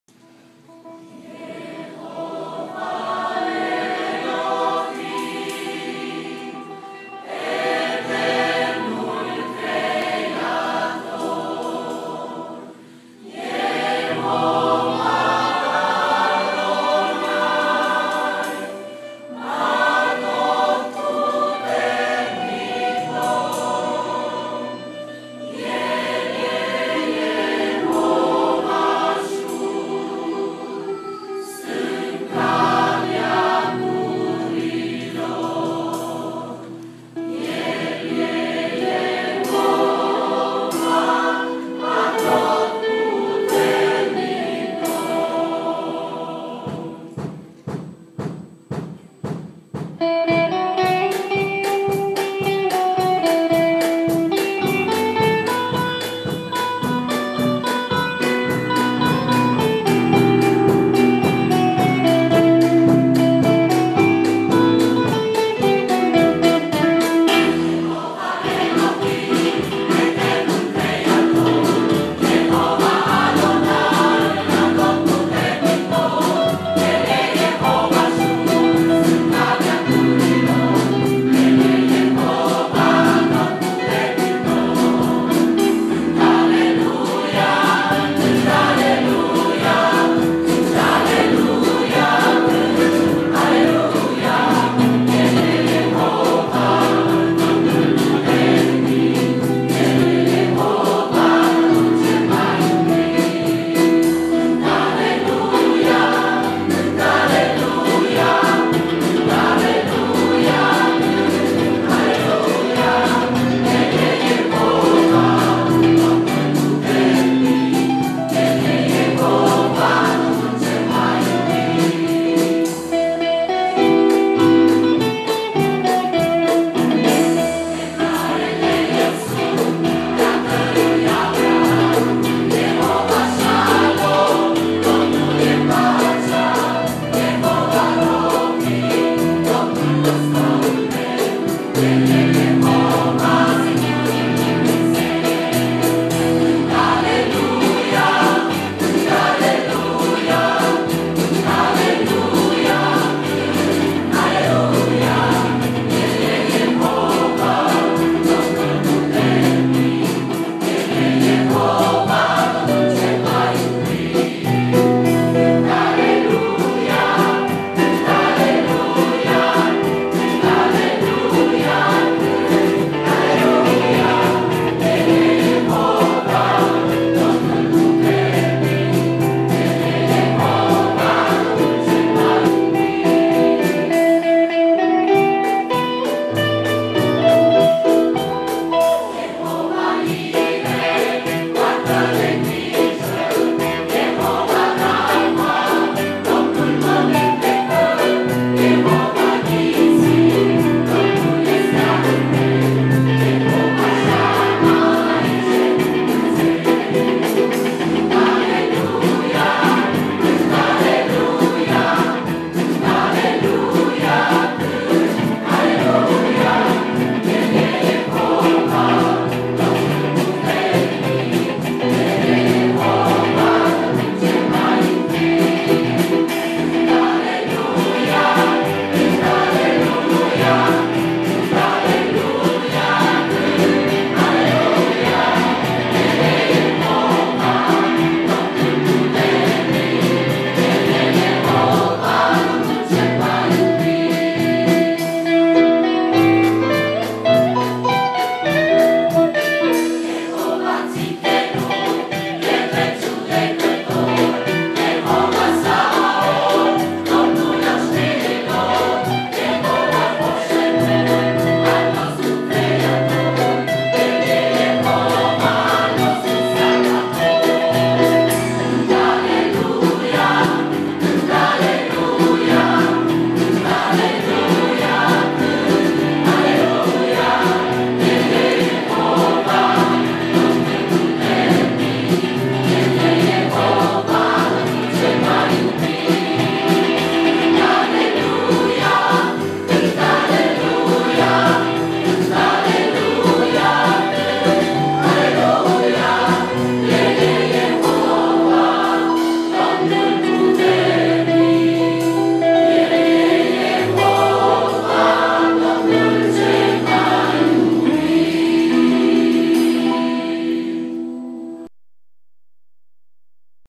Corul Muntele Sionului Iehova
In biserica Elim din Arganda del Rey, Madrid.